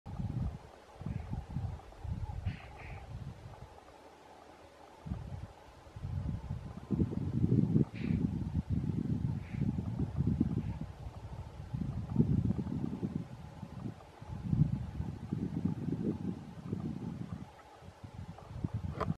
Angry Hartebeest stamps and runs away
Out for a walk on the SA Lombard Nature Reserve and came across a few Hartebeest, who didn't seem to want me around.